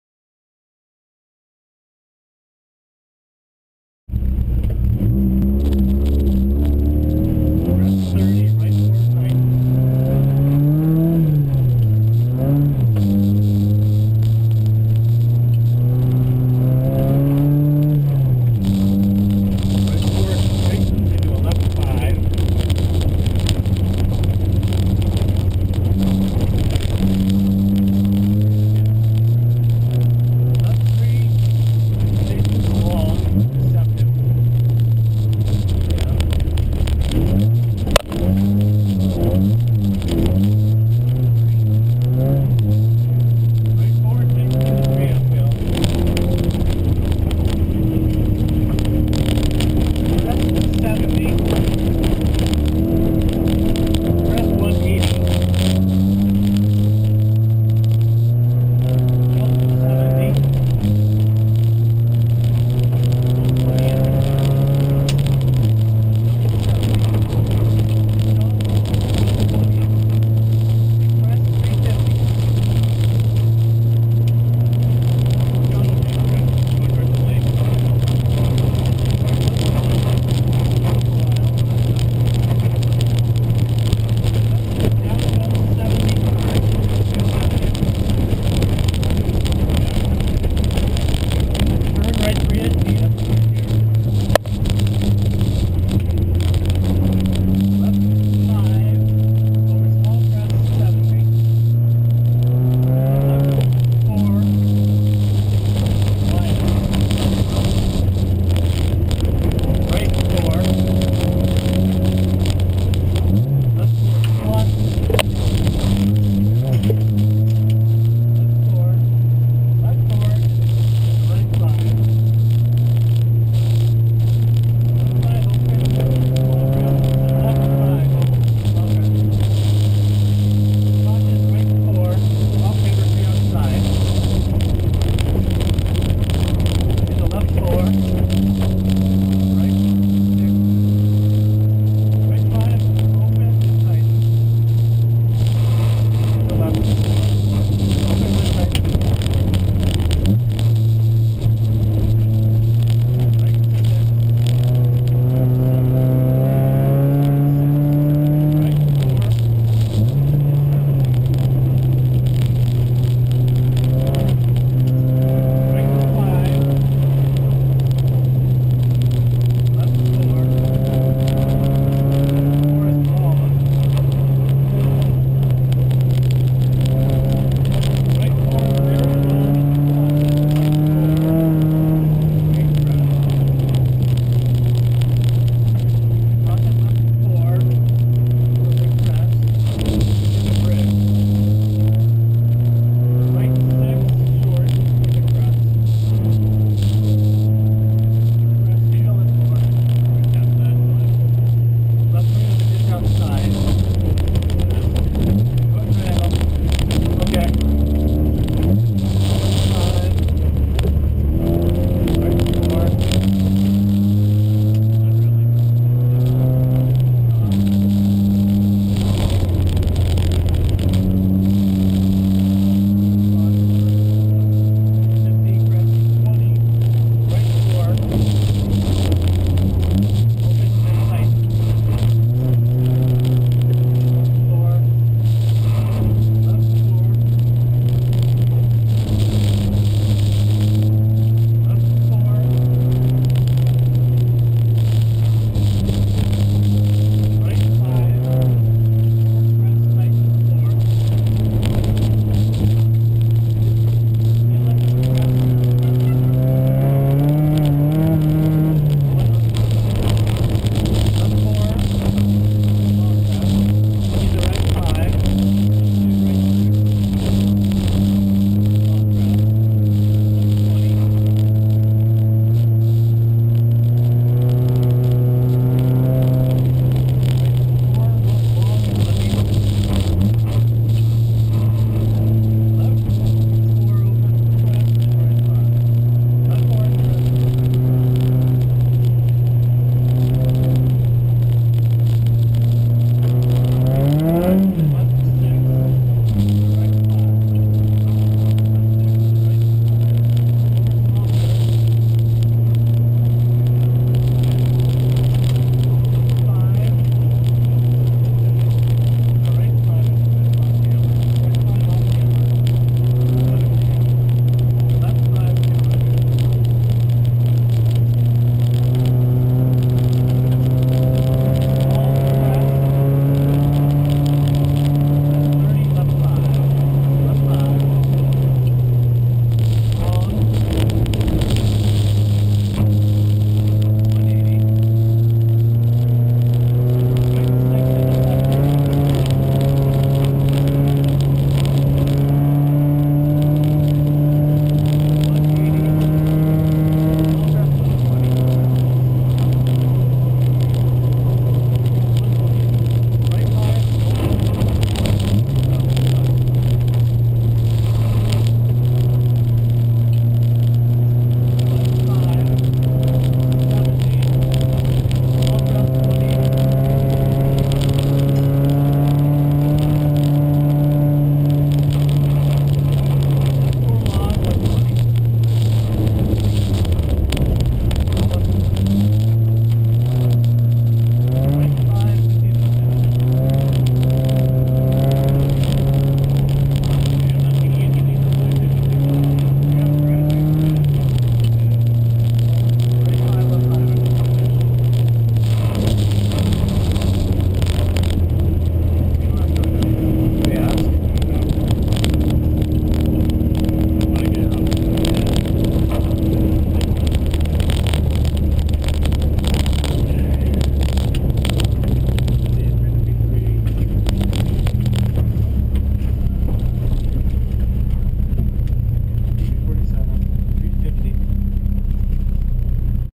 2011 Sno*Drift Rally Stage 2
On-board video from my car on the second stage of the 2011 Sno*Drift Rally.